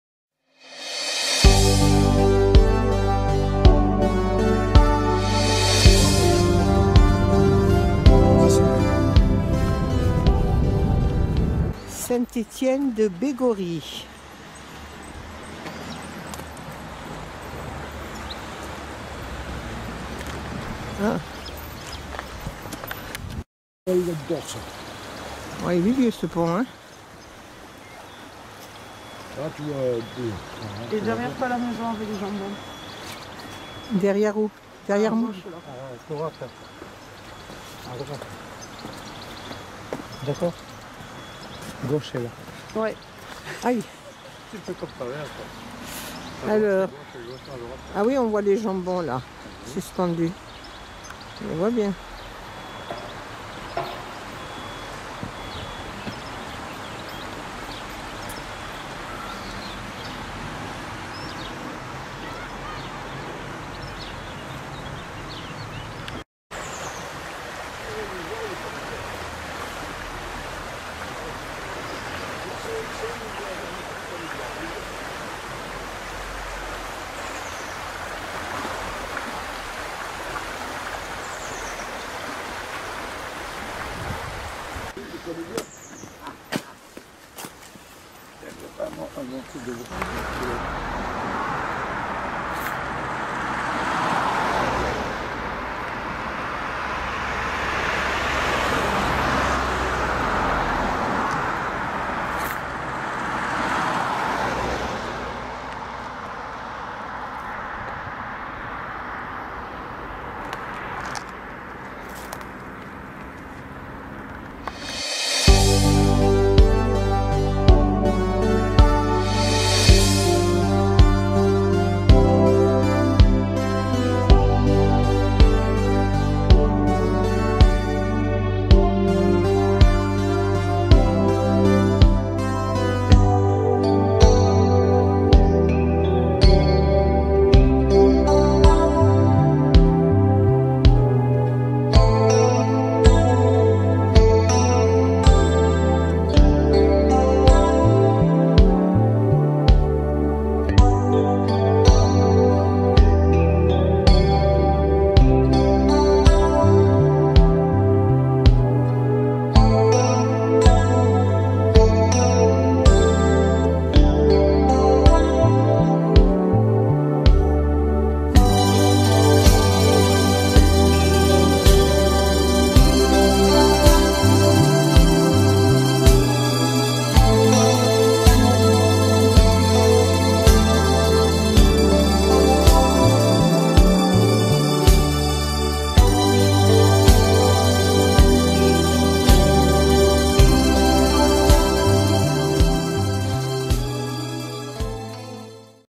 Saint Étienne de BAÏGORRY Avril 2018
Le Pont  romain de  Baïgorry  bénéficie d'une protection et d'un classement aux monuments historiques.
Bien que construit en 1661, il porte l'appellation de " pont romain" car son arche unique en pierre est inspirée de l'architecture romaine.
Le pavage du pont porte encore la marque des charrettes qui l'empruntaient autrefois.
 le château d’ Etchauz  ou Etxauz , début de construction XVIe
Les familles Etxauz sont des proches des rois de Navarre